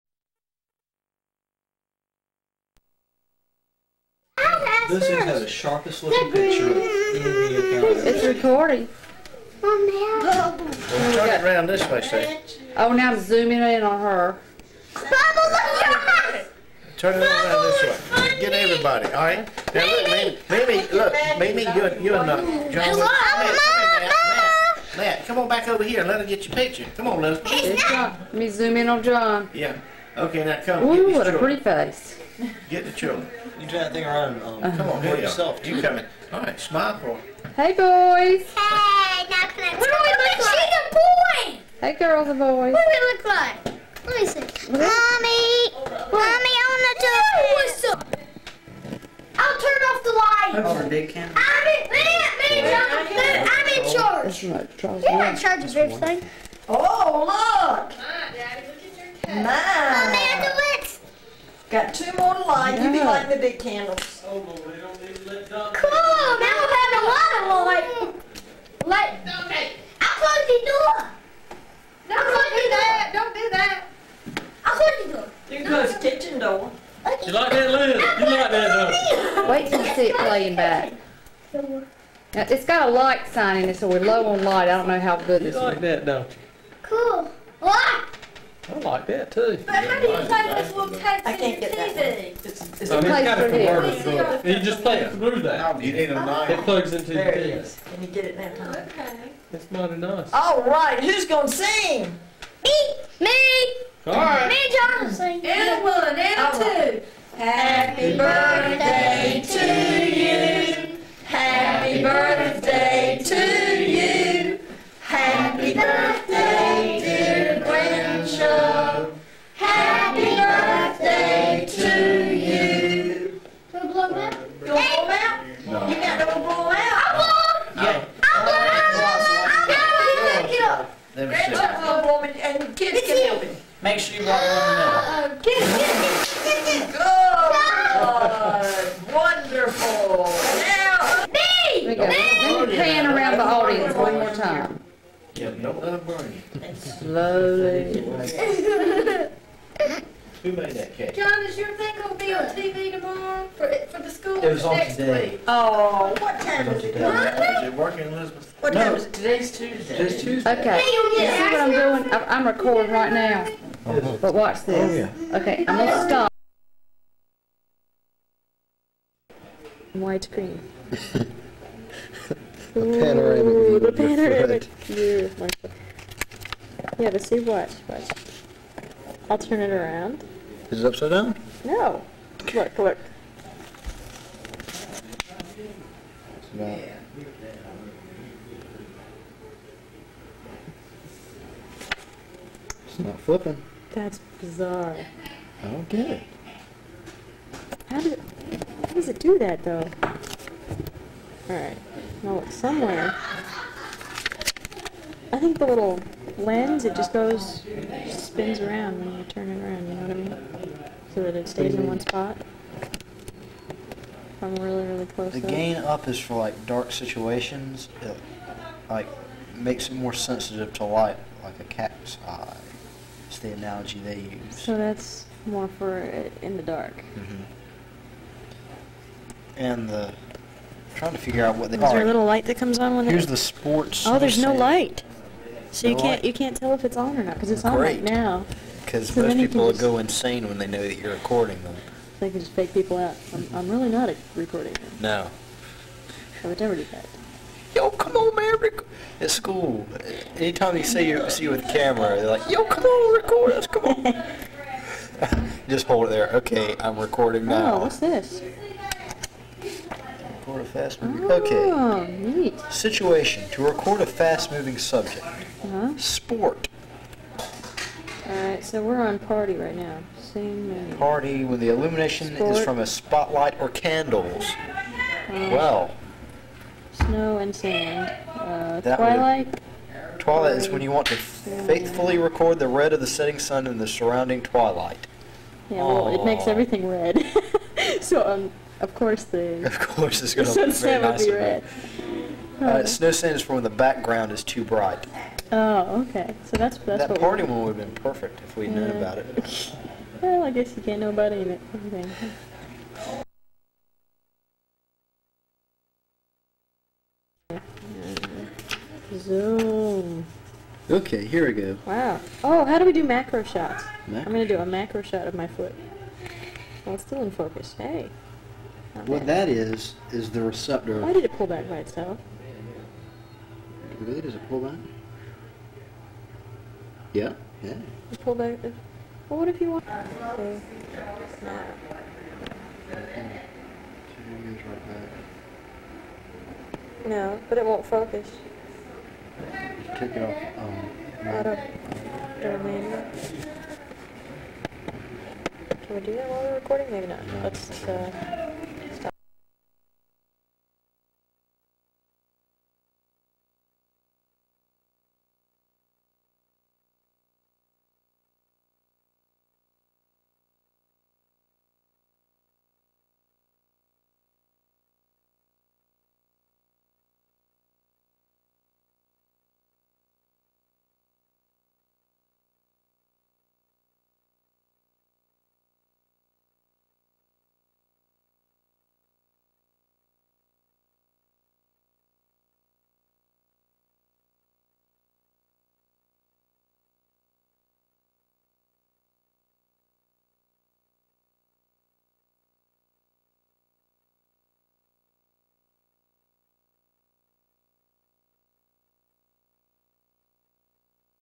Chub's birthday with family by Mystery Bridgers
This is Chubby Bridgers' 80th birthday with family in Goldsboro, NC.